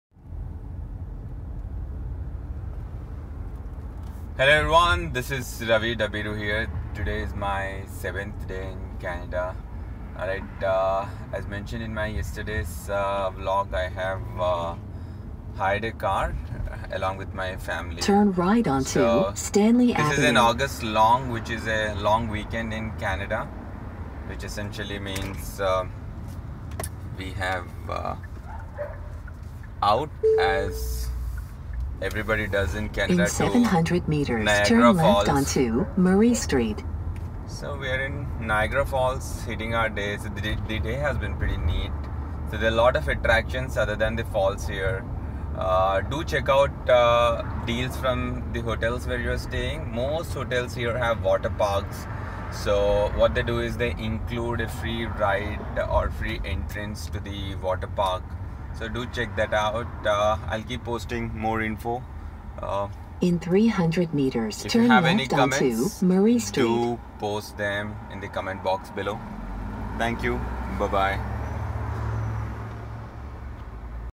Driving and Hiring a Car in Canada on Indian Driving License.
I have hired a Car in Canada on my Indian Driving Lic in Toronto. You do not have to wait for your Canadian Lic to rent/hire a car.

TIP: Lot of people asked me .. do you need International driving permit? No it is not required as long as your DL is in English.